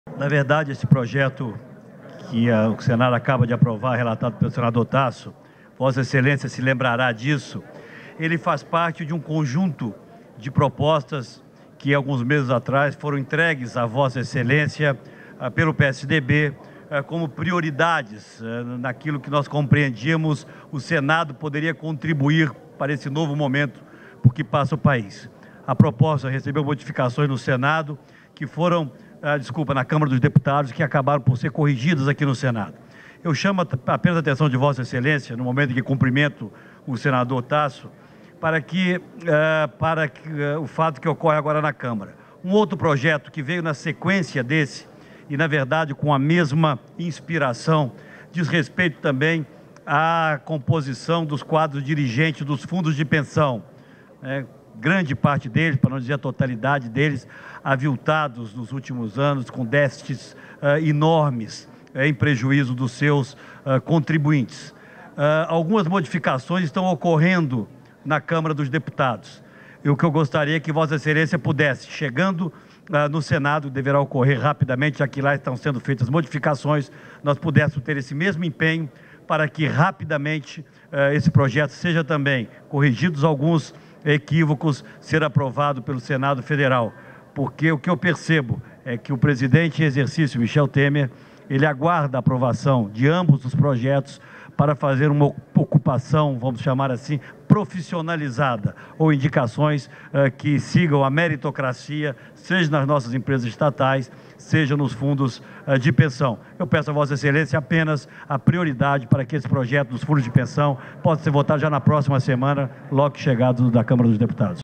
Aécio Neves - Aprovação da Lei das Estatais - 21/06/2016
"Esse projeto que o Senado acaba de aprovar, relatado pelo senador Tasso, faz parte de um conjunto de propostas que, há alguns meses, foi entregue pelo PSDB como prioridades naquilo que nós compreendíamos que o Senado poderia contribuir para esse novo momento por que passa o país”, afirmou o senador Aécio Neves, em pronunciamento, nesta terça-feira (21/06), ao destacar a aprovação do Projeto de Lei 555/2015, que cria a Lei de Responsabilidade das Estatais.

O projeto relatado pelo senador Tasso Jereissati (PSDB/CE) faz parte do conjunto de propostas que foi apresentado pelo PSDB, este ano, como prioridades para votação pelo Congresso.

Em seu pronunciamento, Aécio pediu agilidade à votação do projeto de lei que profissionaliza a gestão dos fundos de pensão das estatais. O projeto teve um substitutivo do senador Aécio Neves já aprovado pelo Senado.